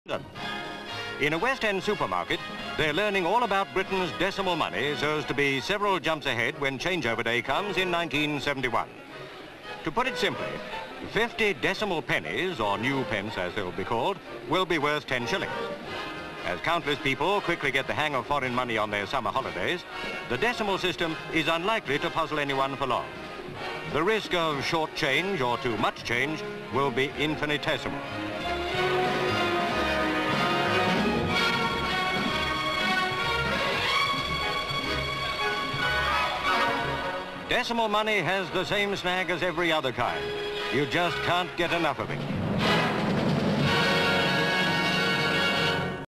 Decimal Currency (1967)
London.

CU. Decimal coins equivalent to ten shillings sterling. GV. People in Berkley Square supermarket, London. CU. Sign 'Coffee nine shillings 45 NP.' SV. Pan, cash desk where sterling is changed for decimal coins. CU. Girl checks conversion table. SV. Pan, customer changing sterling for decimal coins. CU. Girl at cash desk counting out coins, & CU. SV. Goods on shelves marked in decimal system. CU. Eggs with decimal price. CU. Woman takes box of eggs. CU. As she pays in decimal coinage. CU. Pan, another woman looking at her decimal coins. SV. Woman at cash desk totalling up goods.
 FILM ID:2009.34